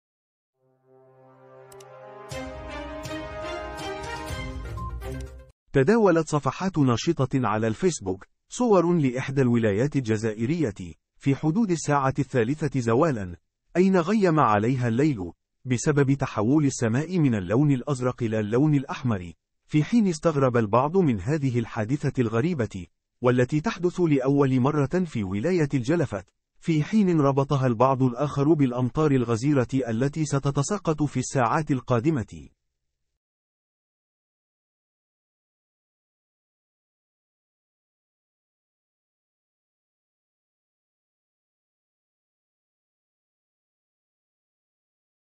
لن تصدق ماذا حدث في بلدية مسعد في ولاية الجلفة ناس كامل دخلت لديارها والسبب... !
لن تصدق ماذا حدث في بلدية مسعد في ولاية الجلفة تحول السماء الى لون أحمر والسبب !